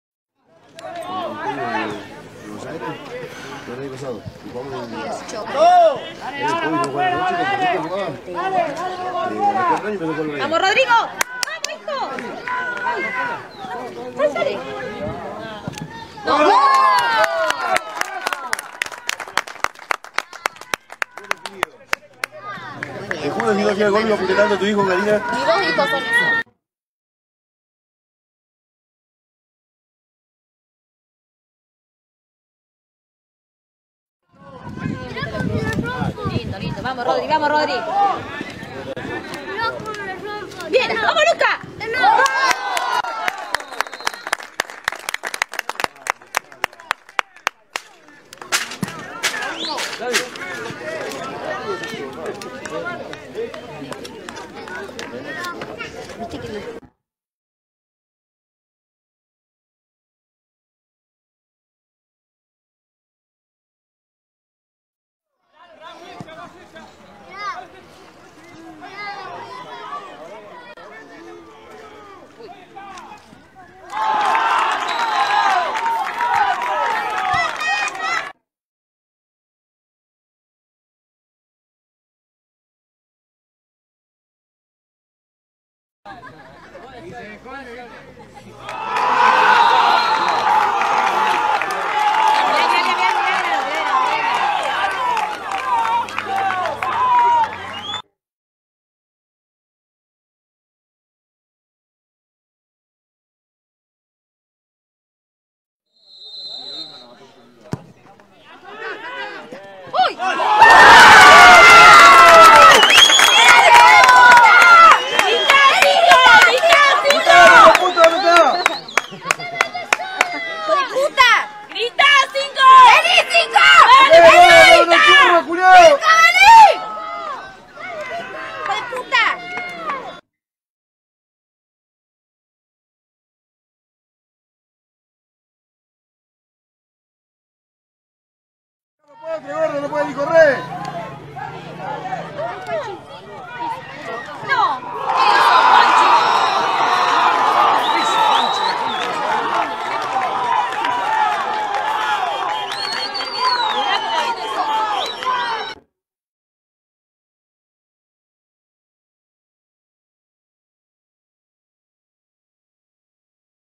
LDFP 2015 Clausura 1era CSR VCP vs PFC VCP
Compacto de goles del partido por la LDFP 2015 Clausura 1era CSR VCP vs PFC VCP
Se incluyen los seis tantos marcados en el encuentro.
Registros originales: Elizabeth Scardigno, co-realizadora de Nuestros Colores Locales. Una contribución a la identidad carlospacense.